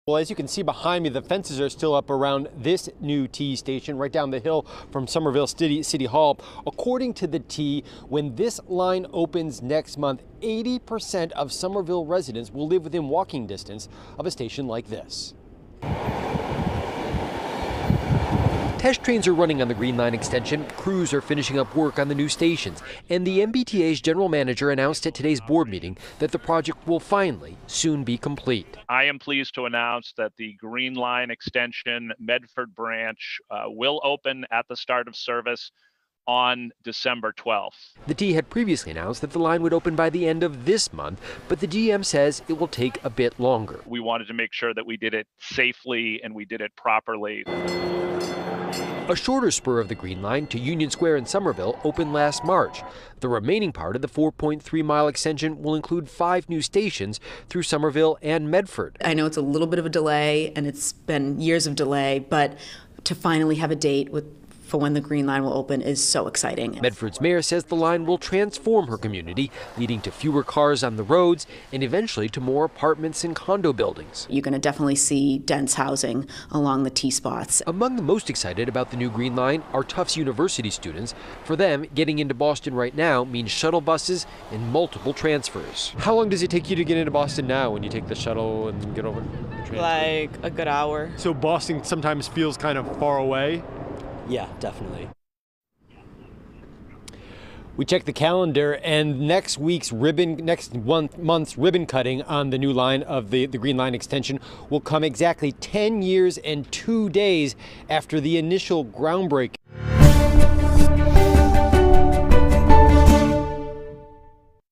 Medford Branch of Green Line Extension to open on Dec. 12
The final ribbon-cutting on the Green Line will come ten years and two days after the project initially broke ground.